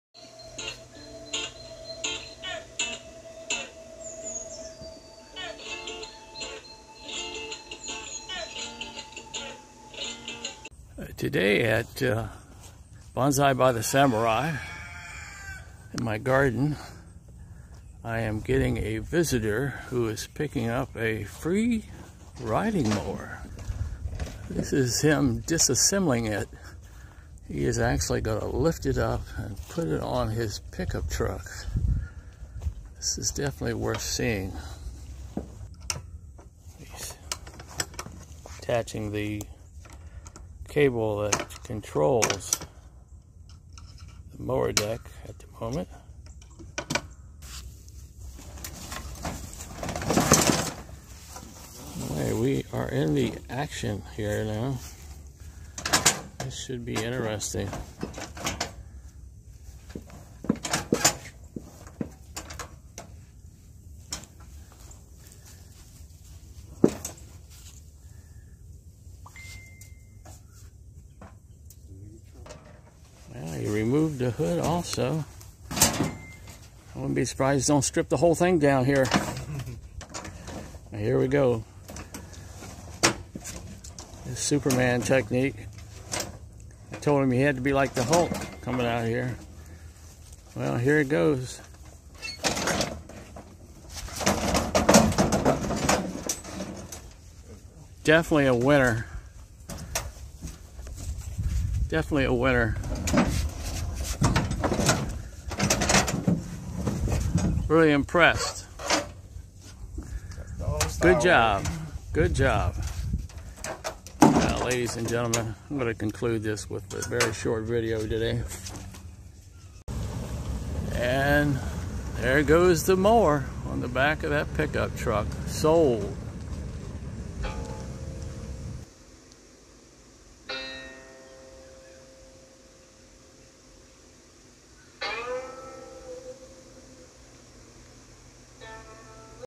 Bonsai by the Samurai, how to lift a riding lawnmower onto a pickup truck @Bonsai by the Samurai
Bonsai by the Samurai, how to lift a riding lawnmower onto the back of a pickup truck @Bonsai by the Samurai